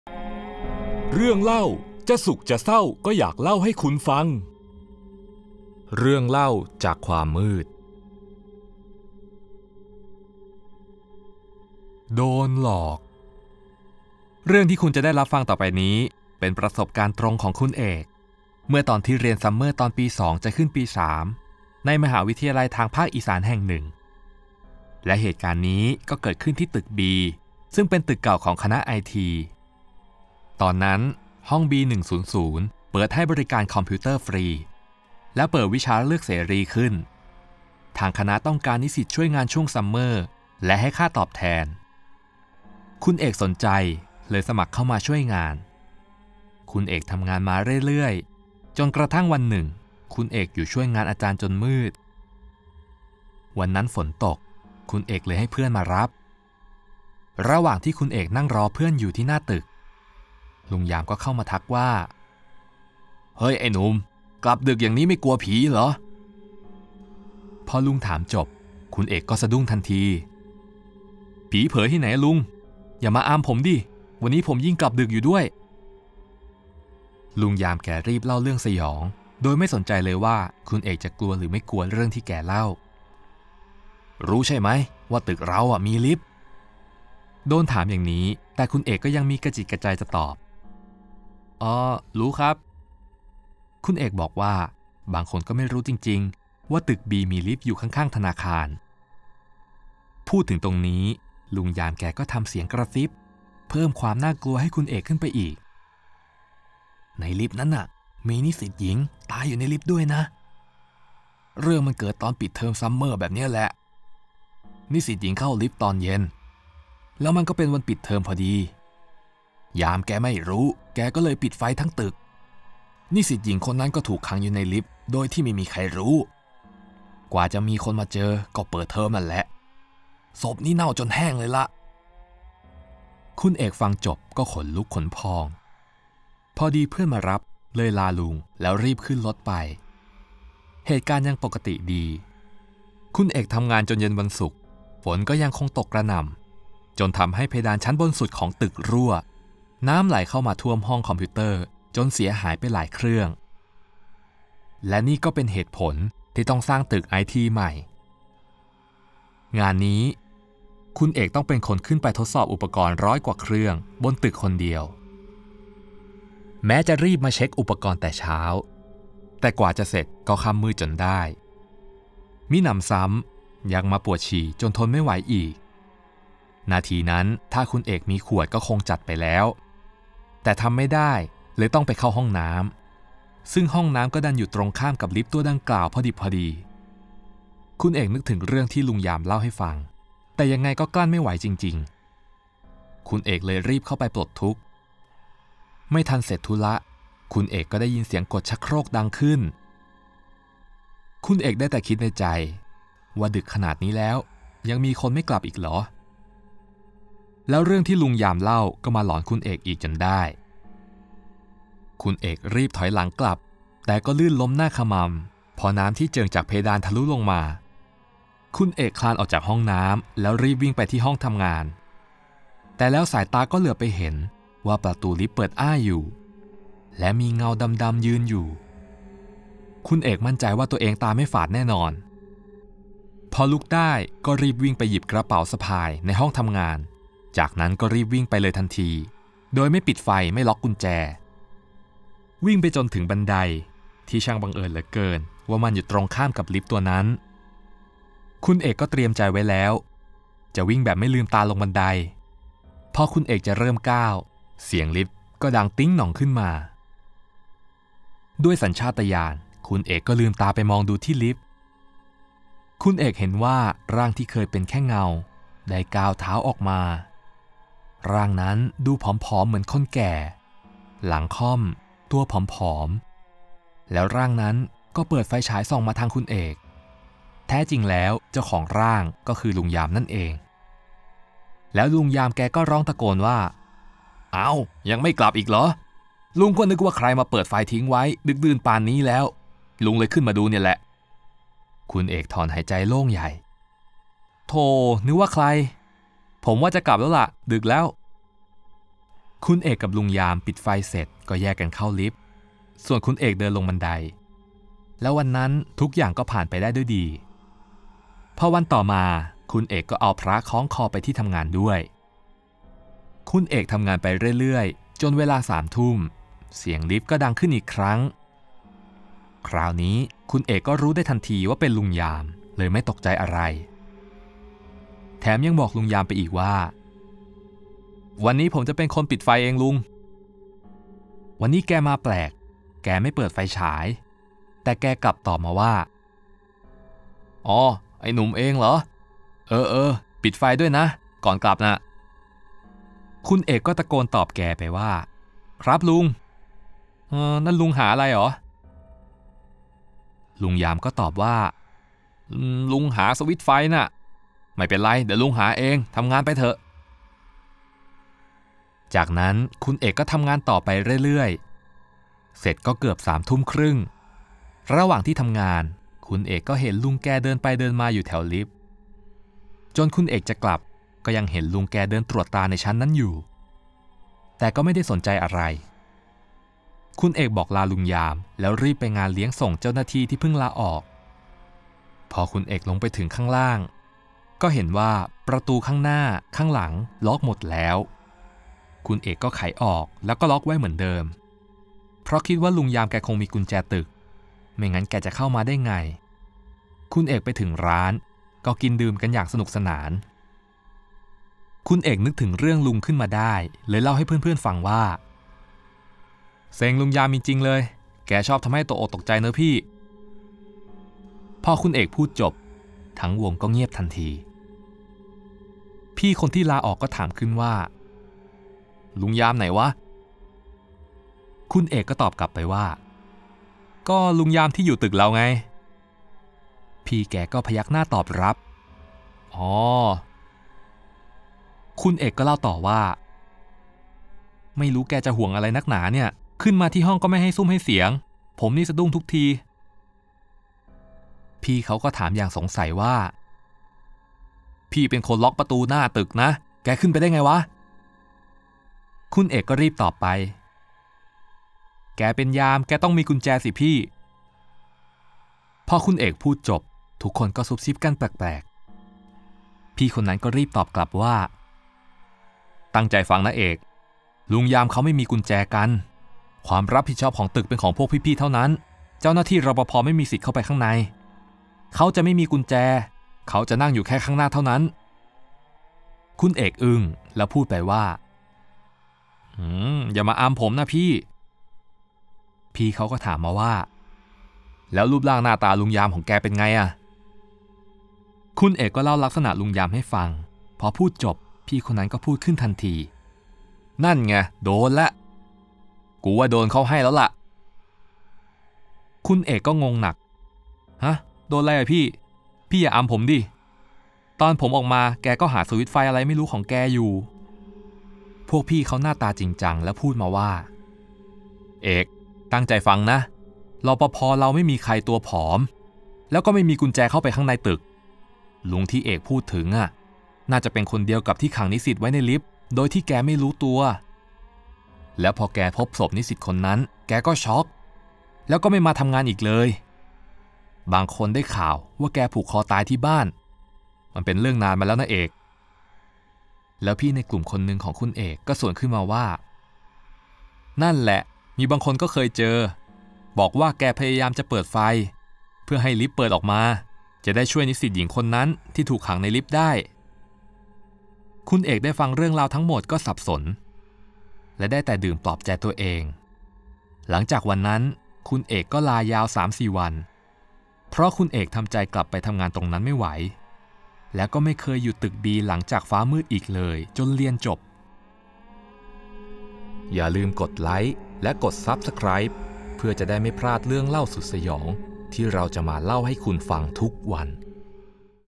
โดนหลอก l เรื่องเล่า
“ในลิฟต์นั่นอะ มีนิสิตหญิงตายอยู่ในลิฟท์ด้วยนะ เรื่องมันเกิดตอนปิดเทอมซัมเมอร์แบบนี้แหละ นิสิตหญิงเข้าลิฟท์ตอนเย็น แล้วมันก็เป็นวันปิดเทอมพอดี ยามแกไม่รู้ แกเลยไปปิดไฟทั้งตึก นิสิตหญิงคนนั้นก็ถูกขังในลิฟต์โดยที่ไม่มีใครรู้ กว่าจะมีคนมาเจอ ก็เปิดเทอมนั่นแหละ ศพนี่เน่าจนแห้งเลยล่ะ” คุณเอกฟังจบก็ขนลุกขนพอง